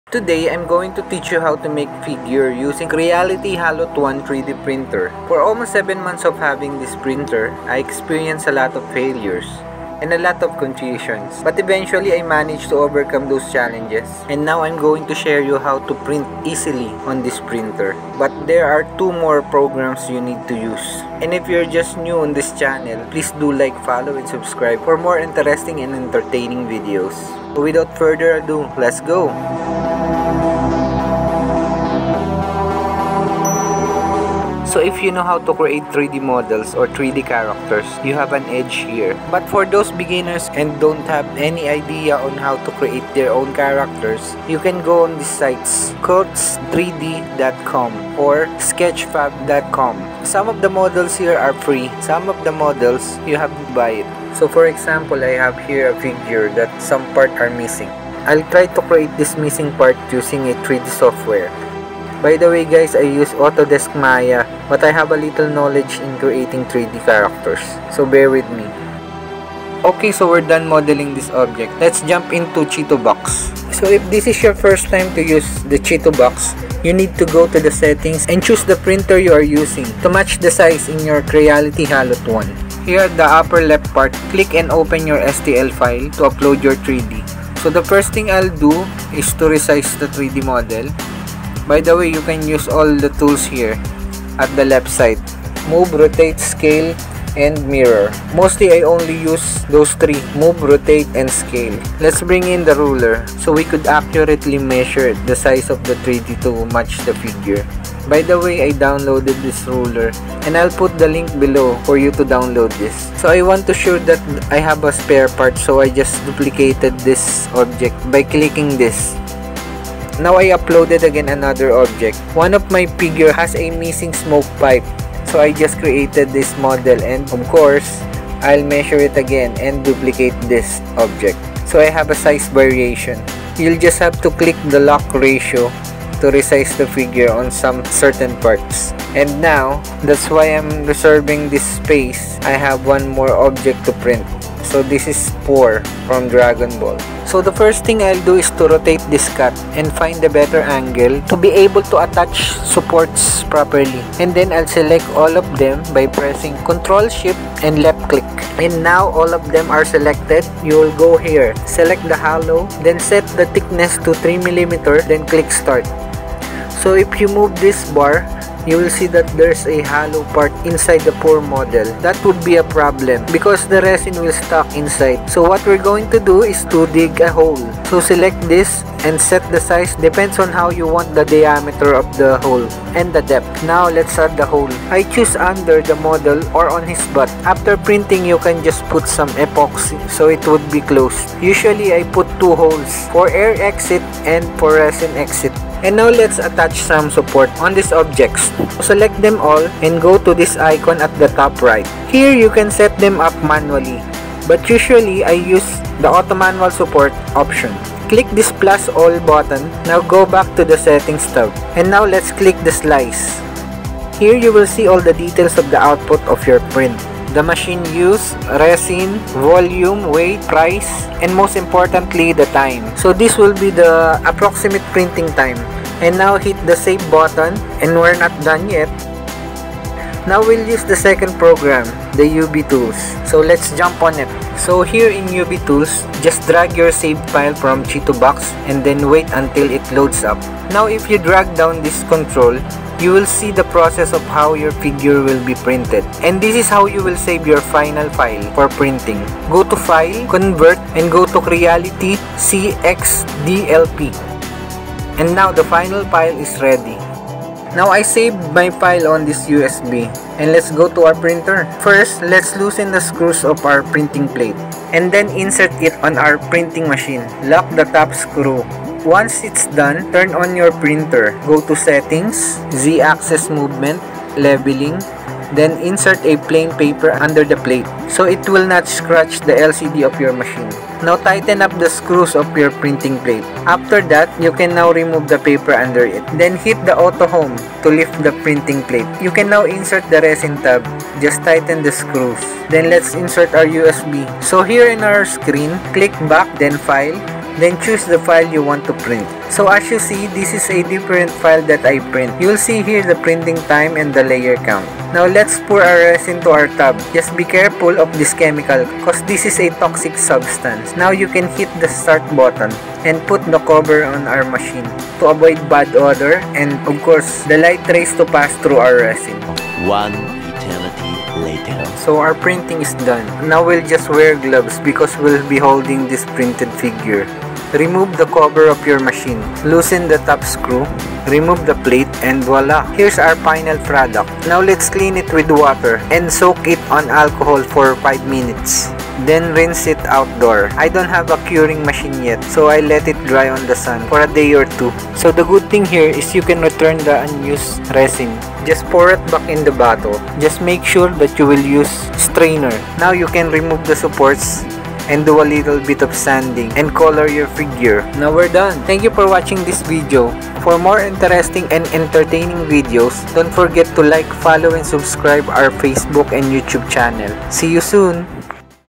HOW TO PRINT FIGURES USING CREALITY HALOT ONE EASIEST STEP BY STEP PROCESS | Gyaru & Neko
In this video we will show you guys , how to print 3d figures using creality one printing machine made of resins.